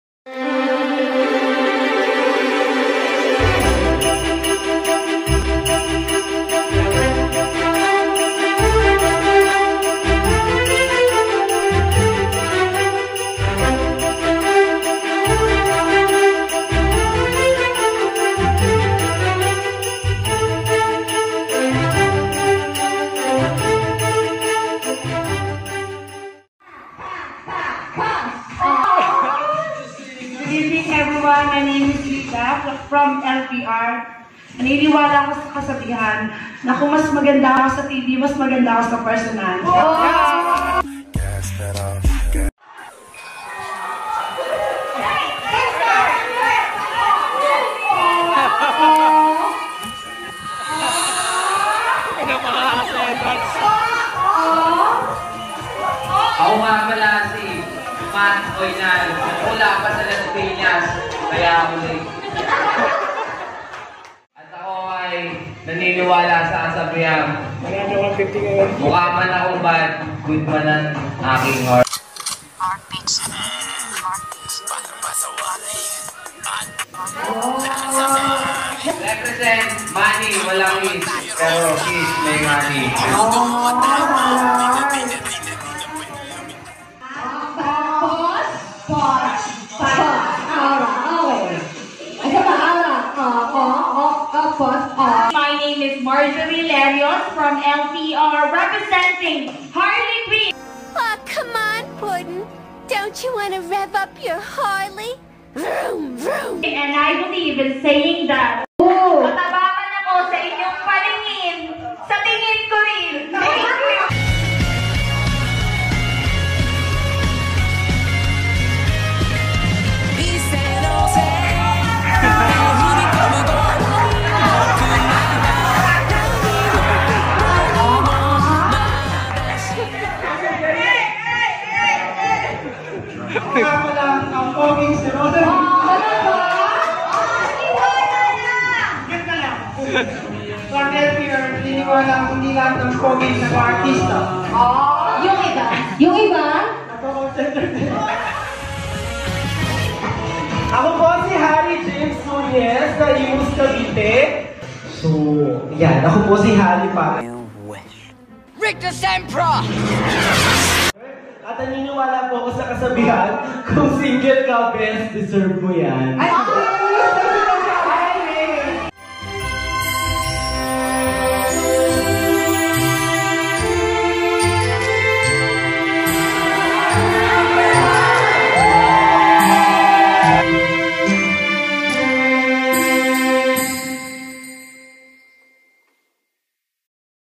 WinSol Christmas Party: Memorable Intros
Mga nakakatawang intro at motto ng mga staff ng WinSol sa aming 2019 Christmas party